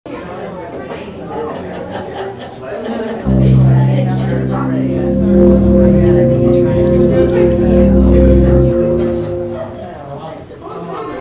BUSINESS FOOD
We all have been there, circled around each other like vultures picking over roadkill at an outback steakhouse, or chilis. We talk about our aspirations and such and pretend it's all for the good of the company but let's be real here this is business food. It is meant for you to eat and zone out the rest of the day and if they try to take my business food away? Well, I'll be bummed and probably have to eat healthy or some shit.